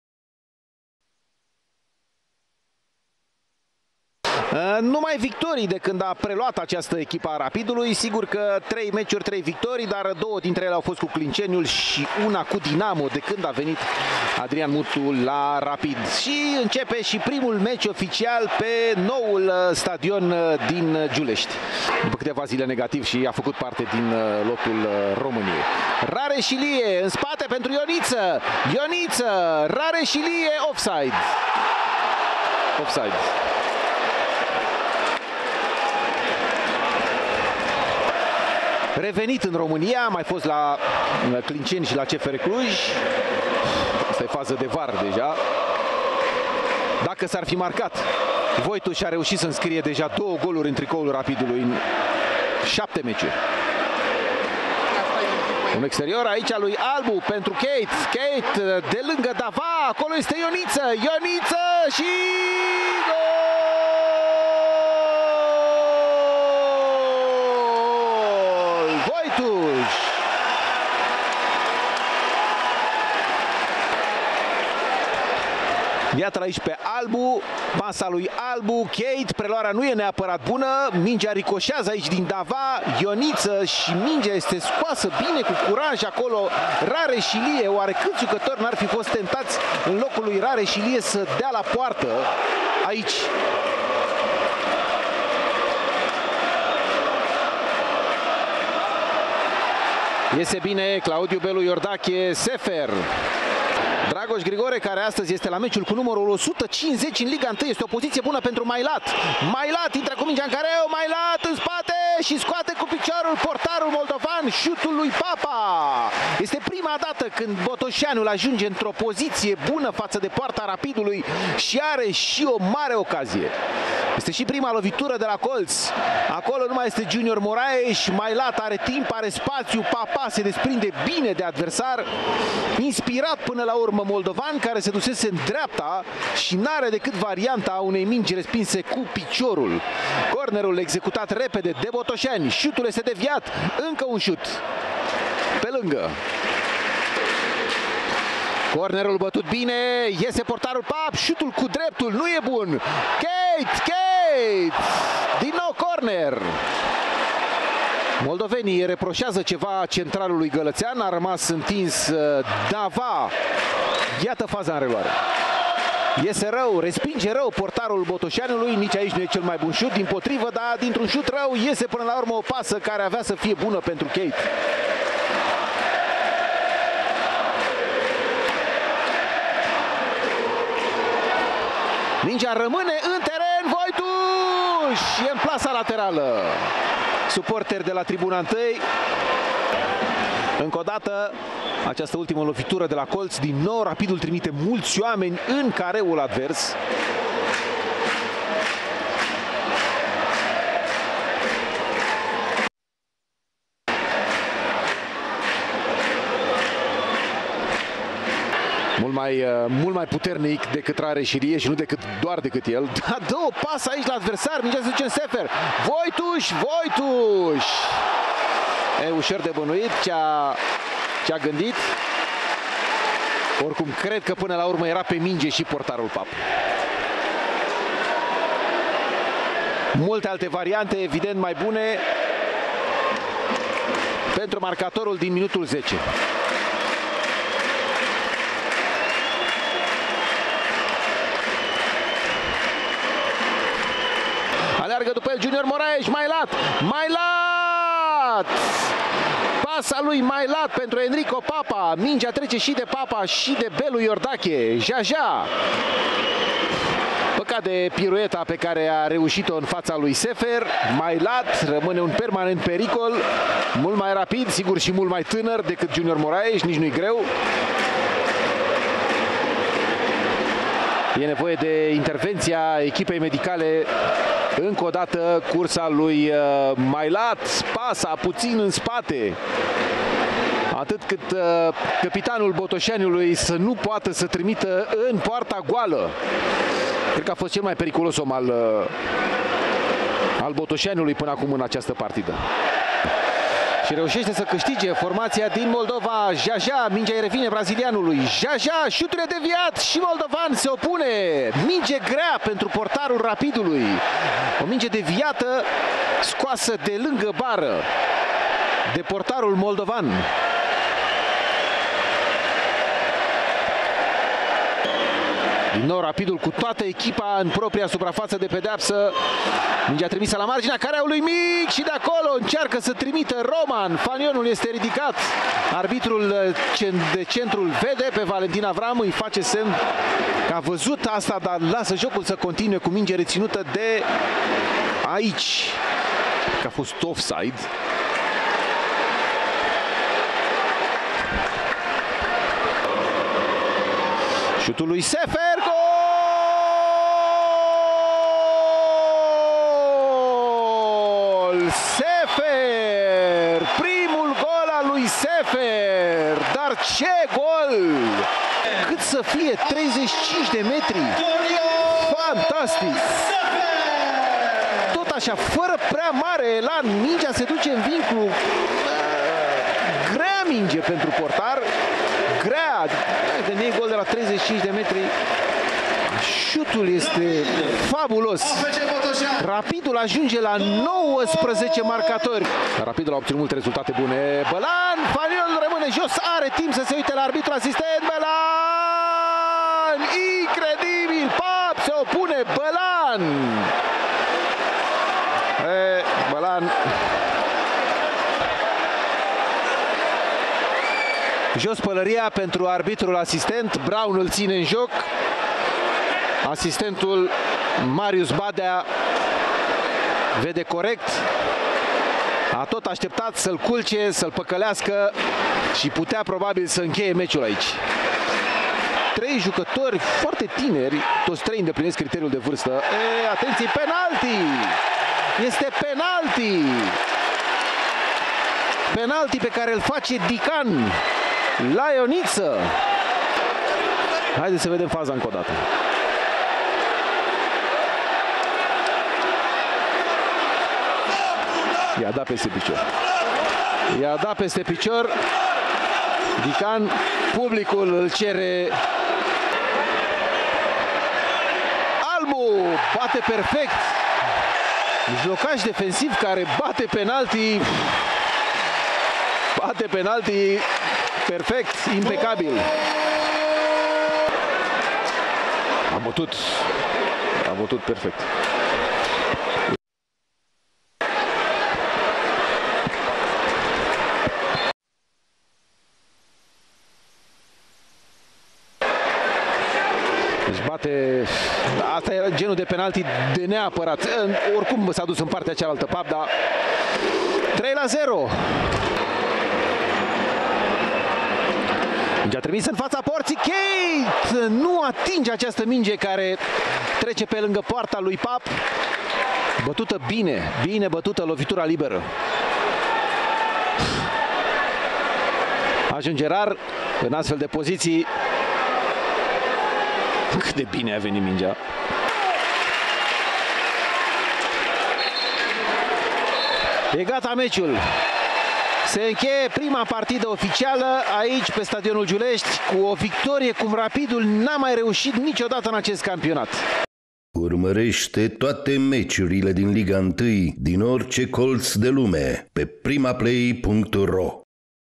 REZUMAT: Rapid - FC Botoşani 3-0. Sărbătoare în Giuleşti
Vojtus şi Sefer (2) au fost marcatorii golurilor.